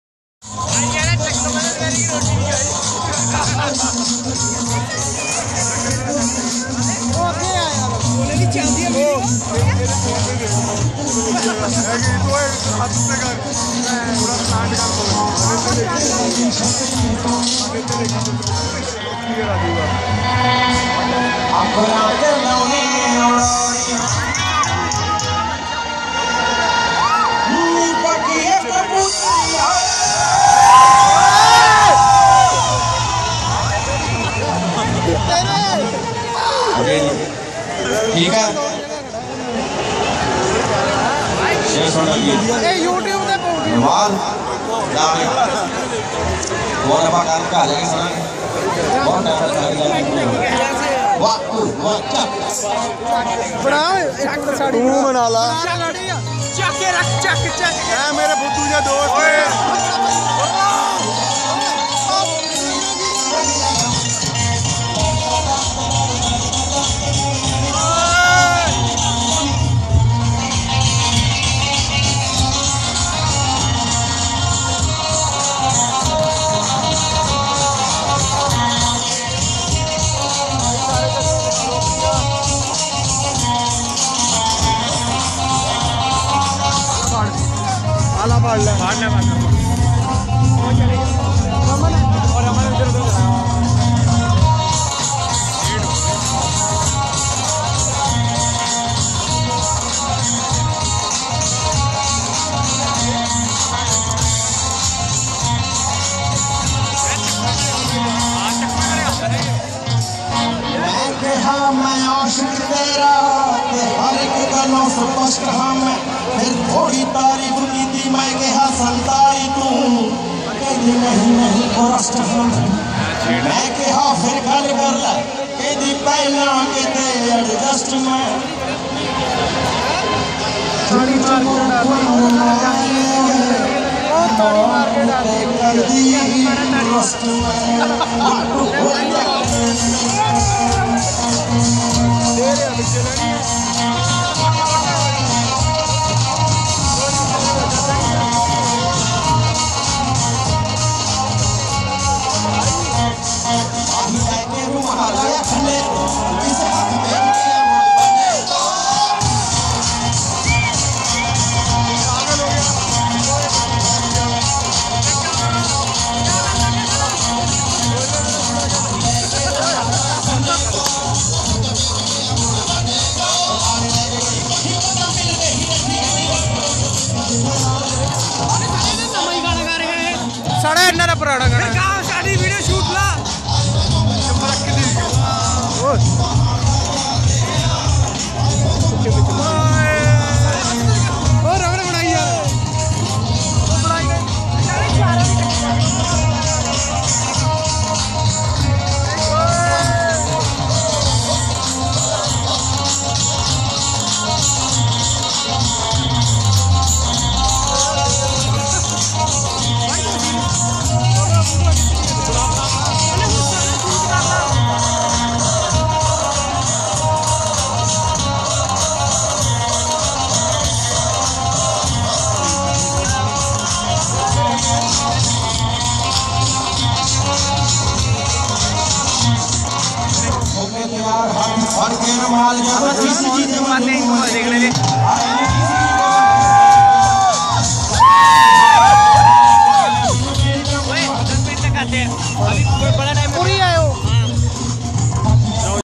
Babbu Maan at Chandigarh Carnival in Leisure Valley, Chandigarh | 29 Nov 2014 | Part 1 | Sheri Pabla
Babbu Maan live during Chandigarh Carnival at Leisure Valley, Sector 10, Chandigarh on 29 Nov 2014..

Recorded by:
Sheri Pabla